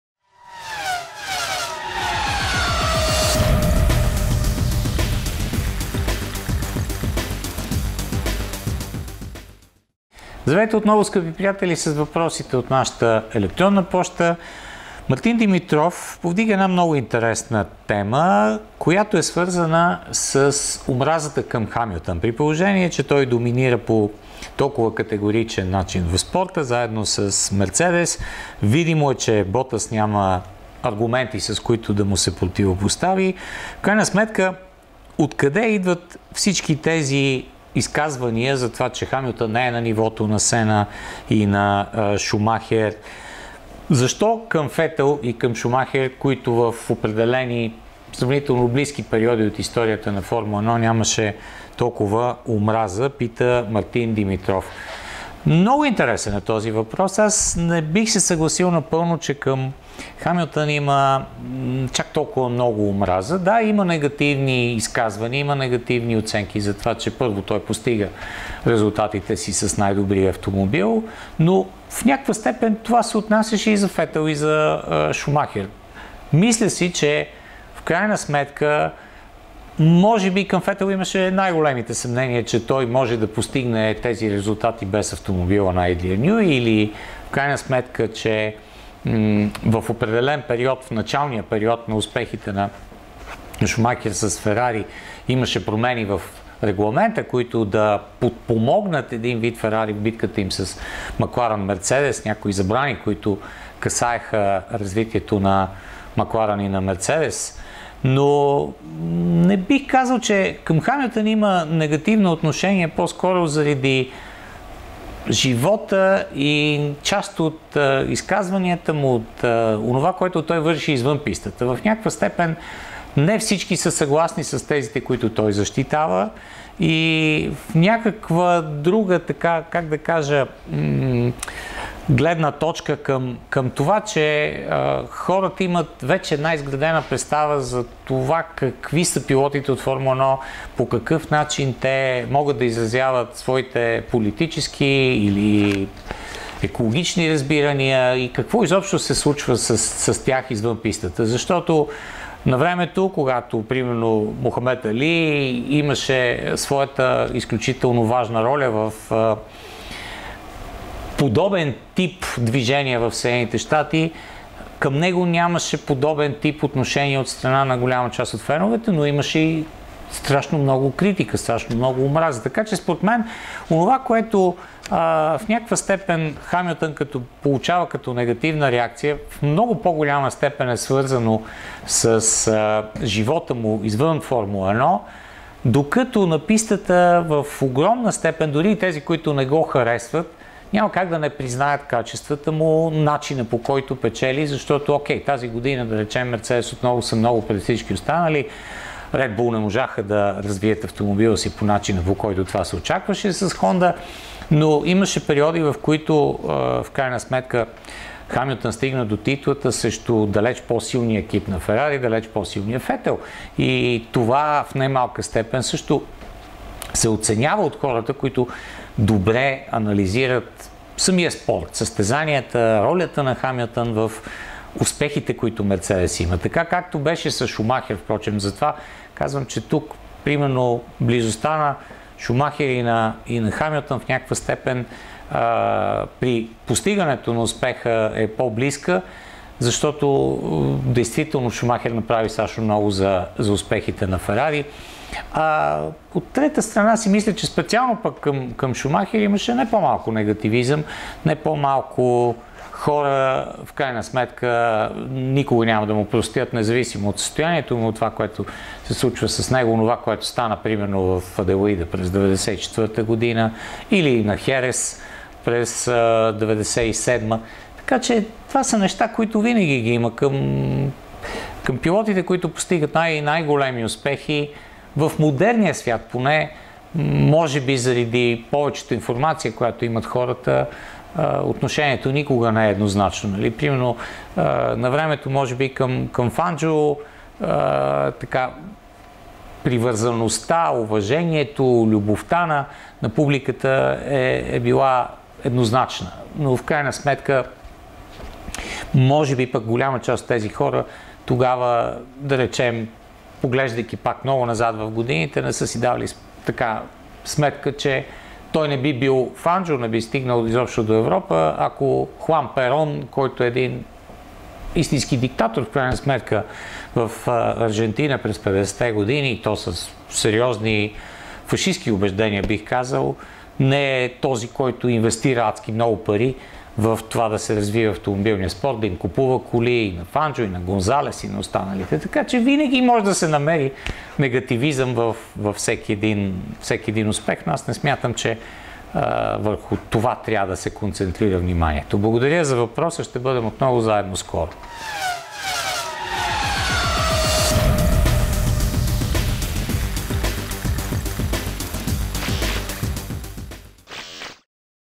Има ли омраза към Хамилтън?
След тази седма поредна титла при конструкторите Мерцедес ли стават най-доминиращият отбор в историята на Формула 1?

Ред Бул имат четири поредни титли, а Ферари шест, въпреки че тогава са имали конкуренция. В тазгодишния шампионат Хамилтън е в своя лига и се е доказал многократно. Ботас не може да го победи четвърта година и дори не може да се доближи до точките.

От къде идва всичката тази омраза и изказвания, че никога няма да е близо до великите Сена и Шумахер при положение, че няма как да се направи сравнение между различни ери.

Защо Фетел или Шумахер не понасяха такава омраза за поредните си титли и доминацията на отборите им спрямо сега.